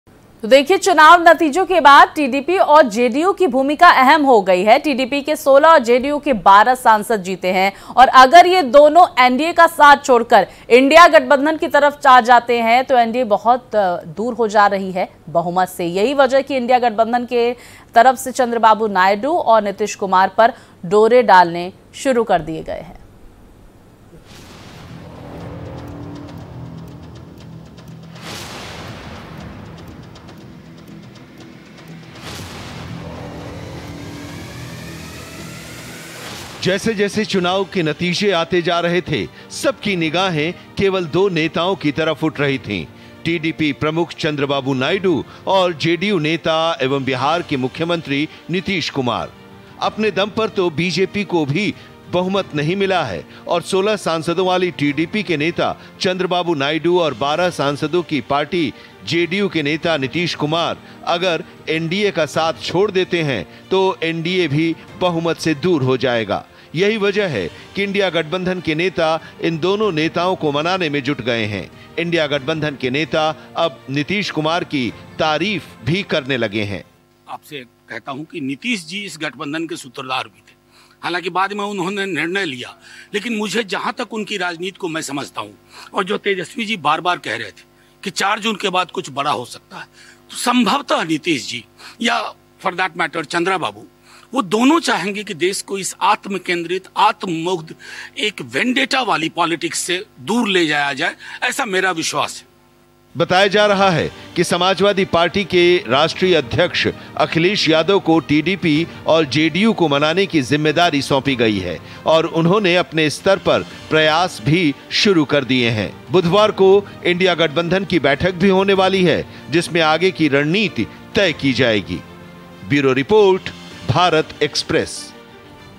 Lok Sabha Election Result : क्या JDU और TDP छोड़ देगी NDA गठबंधन? होने वाला है बड़ा उलट फेर ?
Vandana